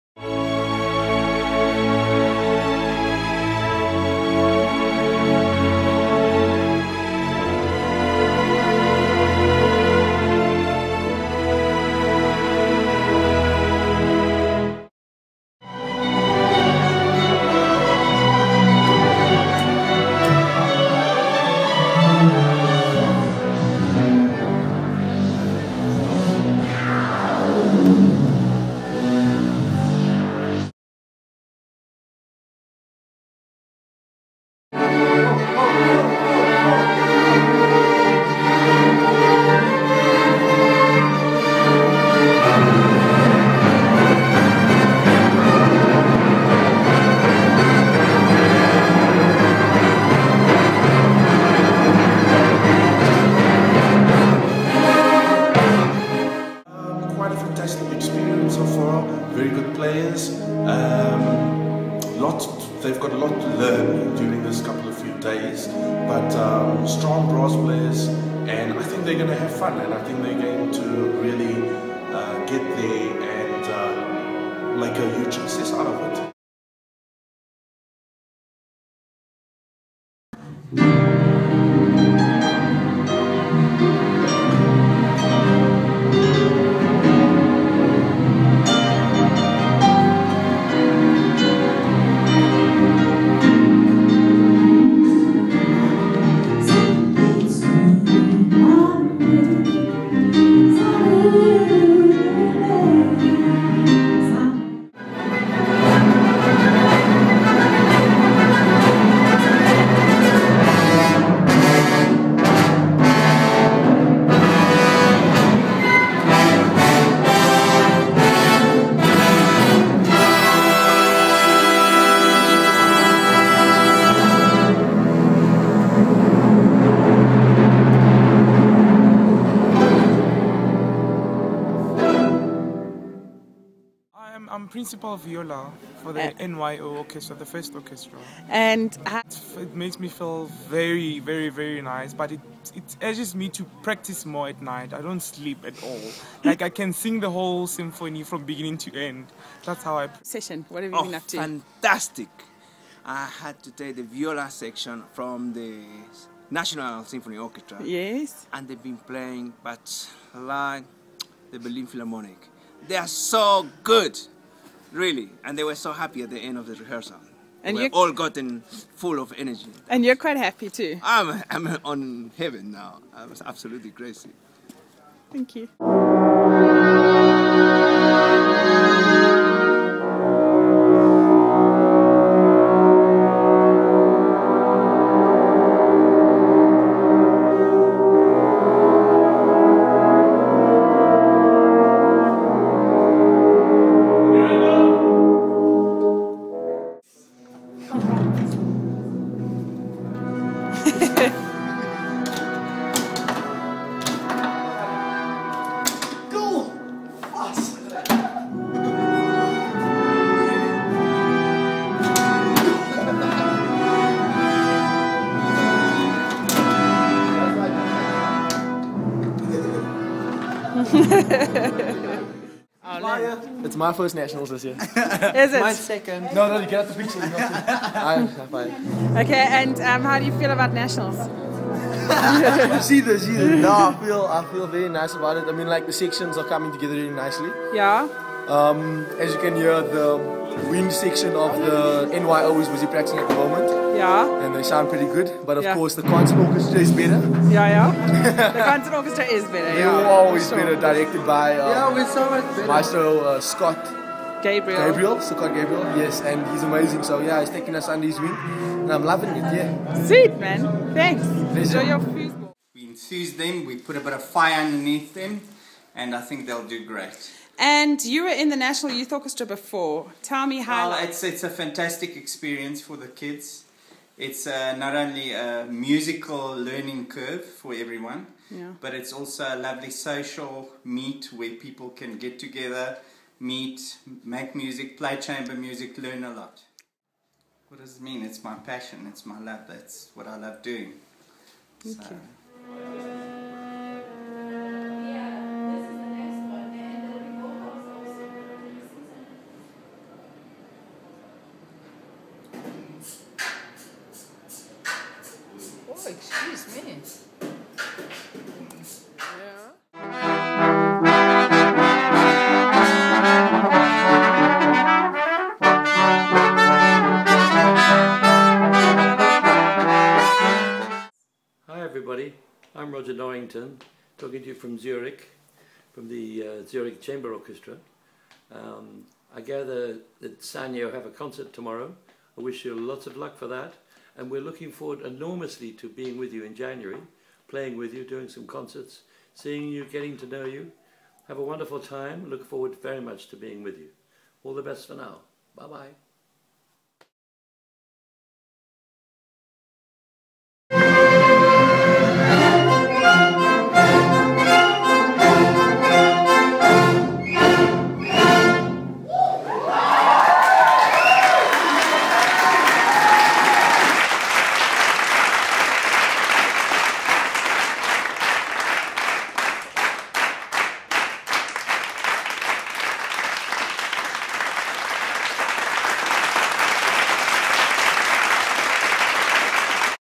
Prep for Nationals 2012
Preparations for the Sasol National Youth Orchestra Course 2012 at Parnassus Farm in the Magaliesberg. A beautiful course in a beautiful place with beautiful music and musicians!